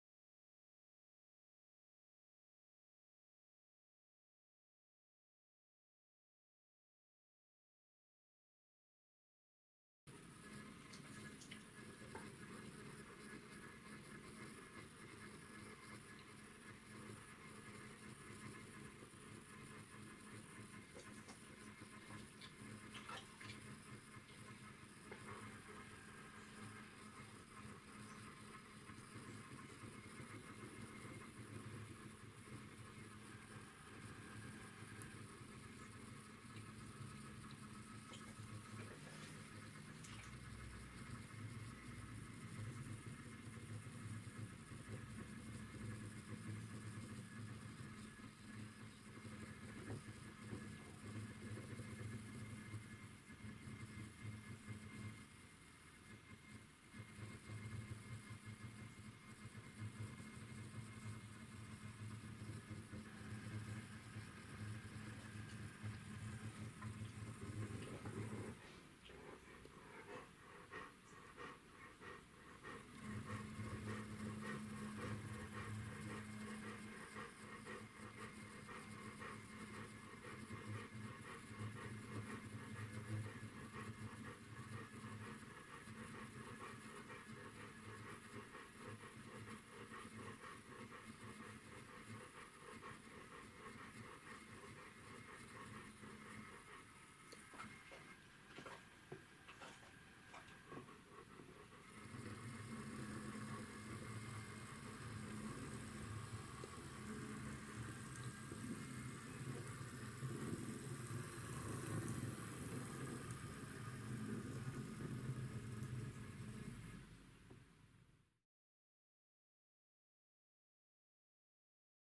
Class 08 decoder tune
Following the last video where I demonstrated the results of converting an old Poole made class 08 shunter to DCC, a few people suggested I needed to tune the decoder by changing a few CV setting, to remove the high frequency whine, and to give slightly smoother slow speed running.  So after an hour or so of programming and testing this is the final result.

As mentioned in the video, this is an old 3 pole open frame motor, but it performs every bit as good as a new multi-pole can motor fitted to todays locos.